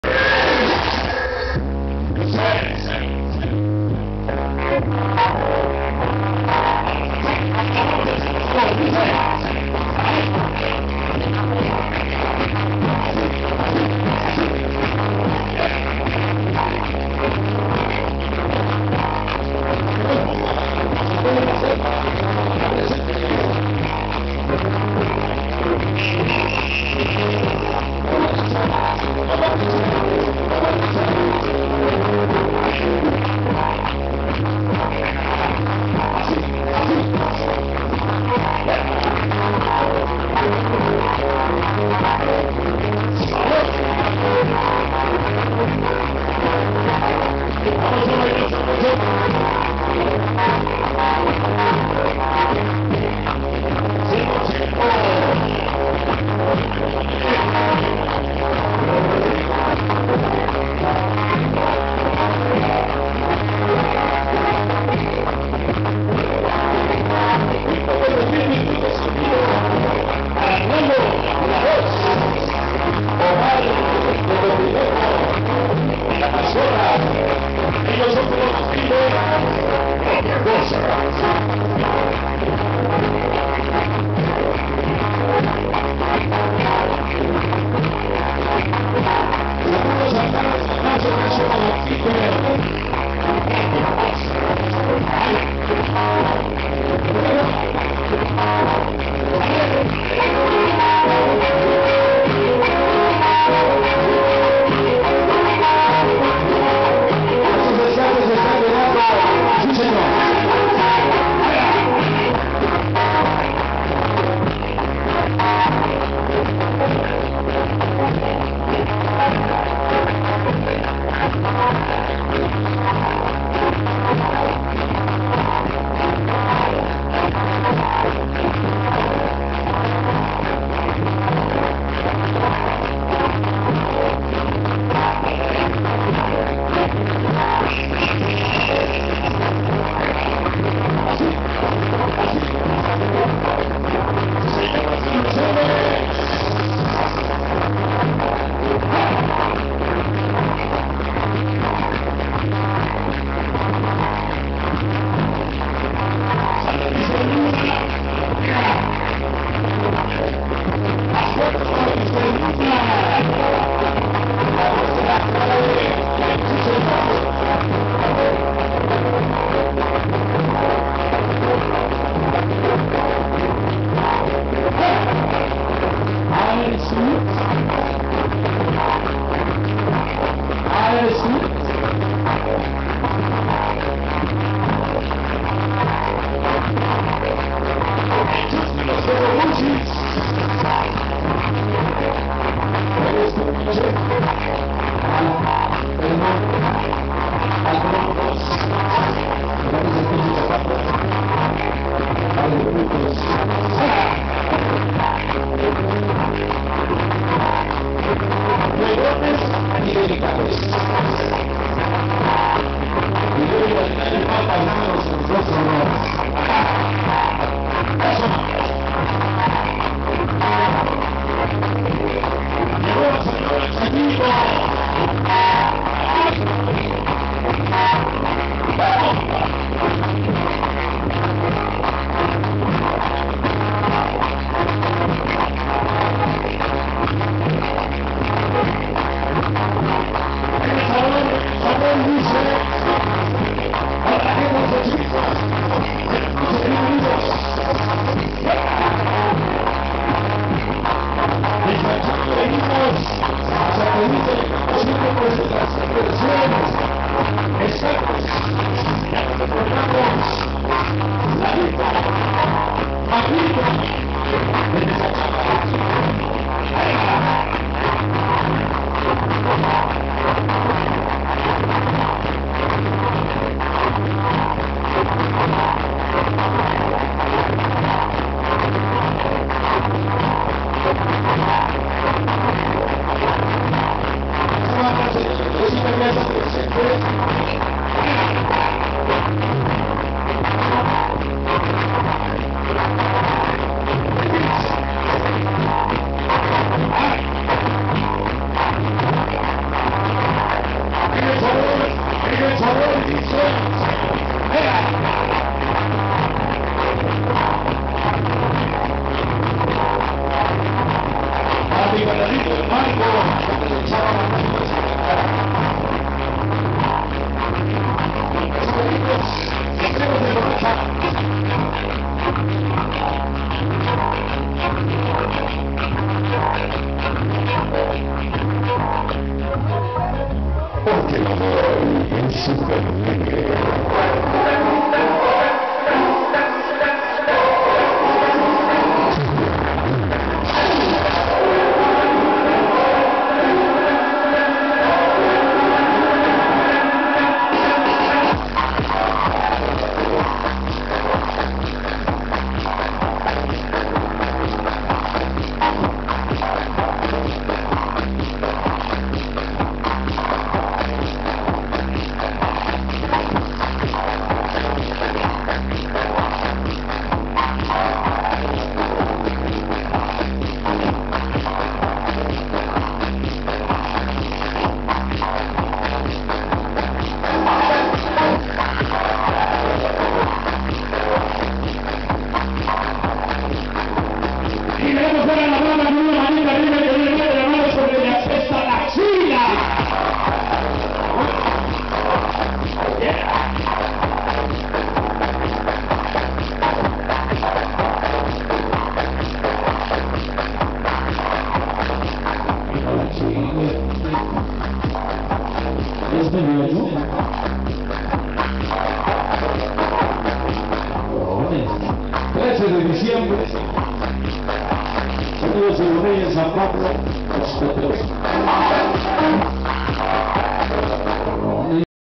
19 DE MAYO EN SAN PABLO OZTOTEPE CON SONIDO SUPER DENGUE
EN BAILE DE FERIA ANUAL CON  RATON MIX ORG JETTA BLANCO RAMON JB YESENIA COROGLASS Y CESAR EL TIO SPERMIK